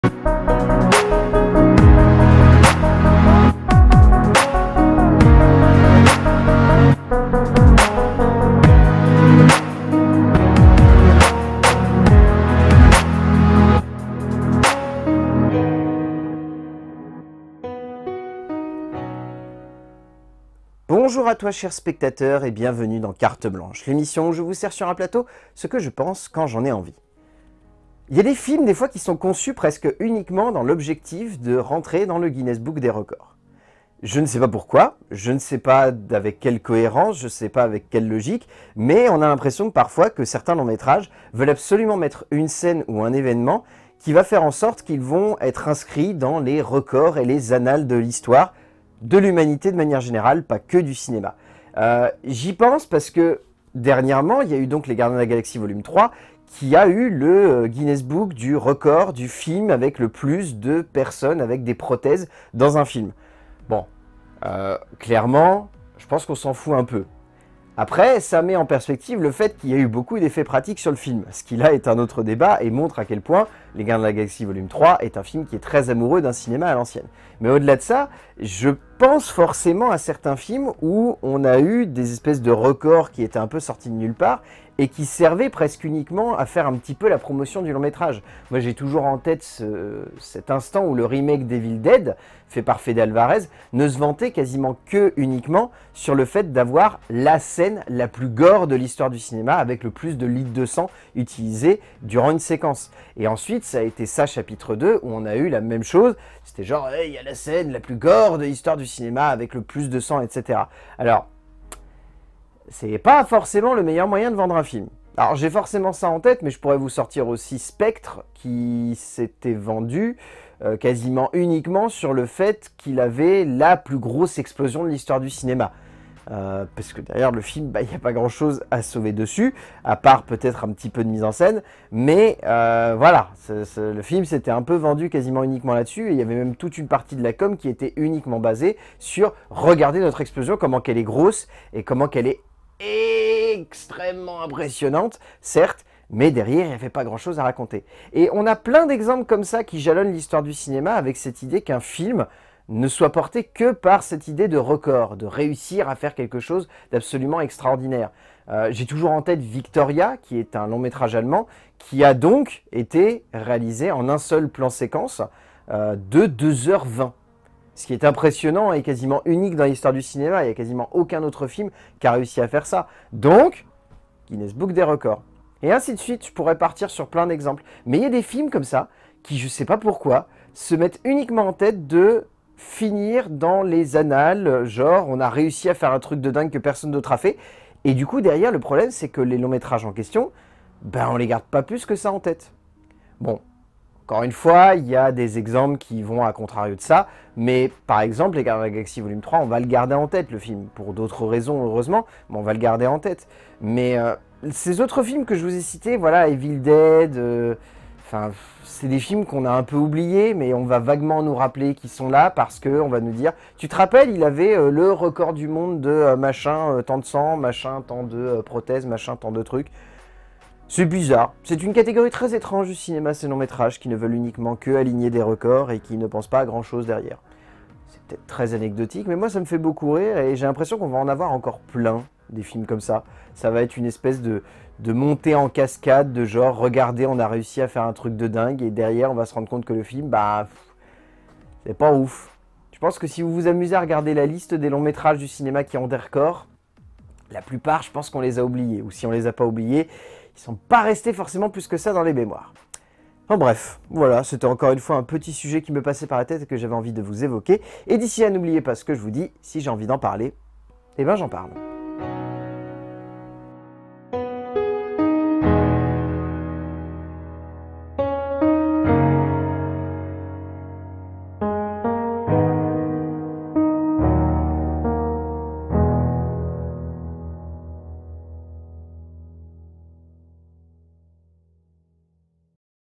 CARTE BLANCHE épisode 20 : les films à "records"
Voici donc le concept du mercredi de l'année 2023, ou je me contenterais simplement de vous parler d'un sujet personnel, dans l'actualité, ou qui m'intéresse, de manière totalement libre
Parlons donc aujourd'hui, d'un type de longs métrages de cinéma assez surprenants, ceux qui ne cherchent pas forcément à être créatifs, mais surtout, à établir un record